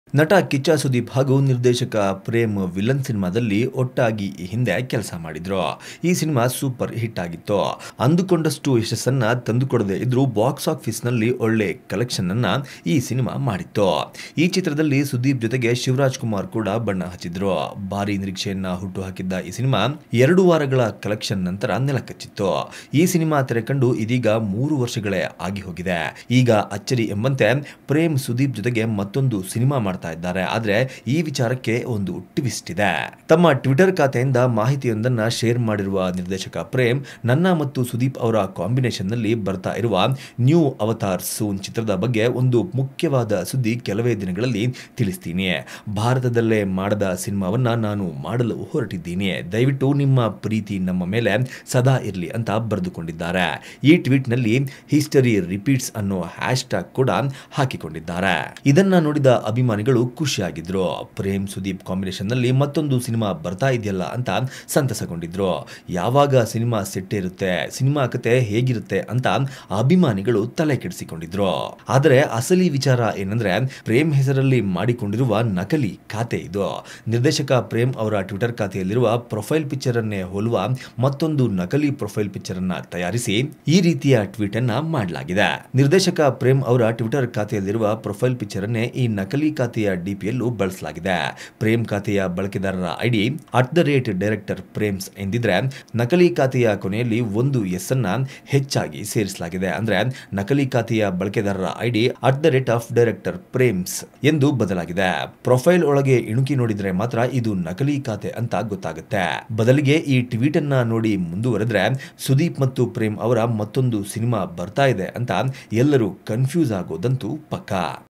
Sudeep ಜೊತೆ ಭಾರತದಲ್ಲಿ ಯಾರು ಮಾಡದ ಸಿನಿಮಾ ಮಾಡ್ತೀನಿ ಅಂದ್ರು ಪ್ರೇಮ್..!? | Filmibeat Kannada
ನಿರ್ದೇಶಕ ಪ್ರೇಮ್ ಅವರ ಫೊಟೋ ಉಪಯೋಗಿಸಿಕೊಂಡು ನಕಲಿ ಟ್ವಿಟ್ಟರ್ ಖಾತೆ ತೆರೆದು ಫೇಕ್ ನ್ಯೂಸ್ ಹರಡಿಸಿದ ಕಿಡಿಗೇಡಿಗಳು
Kannada director Prem fake twitter profile spread fake news